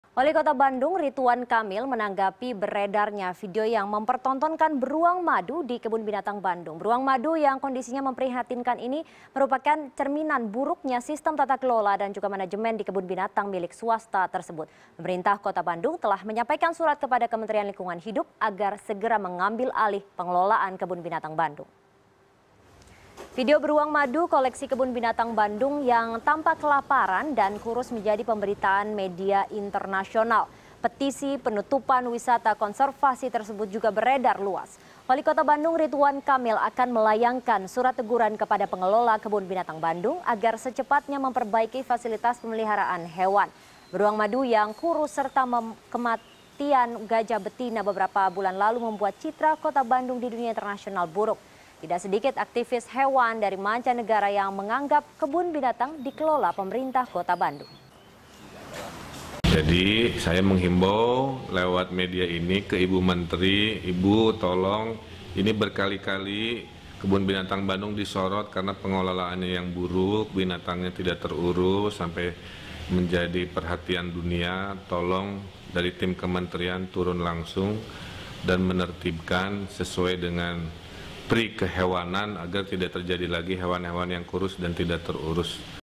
Bonbin Bandung jadi Sorotan, Pemkot Sudah Kirim Surat Teguran ke Pengelola
Wali Kota Bandung, Ridwan Kamil, menanggapi beredarnya video yang mempertontonkan Beruang Madu di Kebun Binatang Bandung. Beruang madu yang kondisinya memprihatinkan tersebut, merupakan cerminan buruknya sistem tata kelola dan manajemen di kebun binatang milik swasta itu. Pemerintah Kota Bandung telah menyampaikan surat kepada Kementerian Lingkungan Hidup, agar segera mengambil alih pengelolaan Kebun Binatang Bandung.